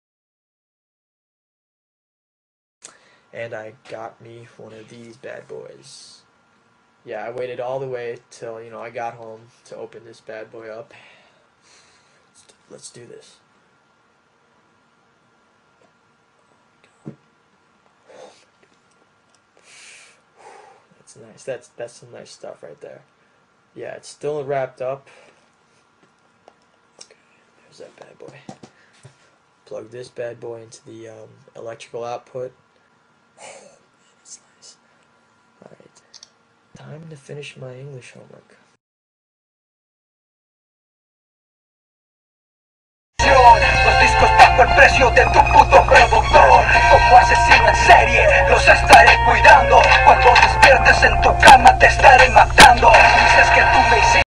The Badboy Count
How many times can Dominic Fajardo say "Badboy"???
They might as well have called the iPhone, the "Badboy"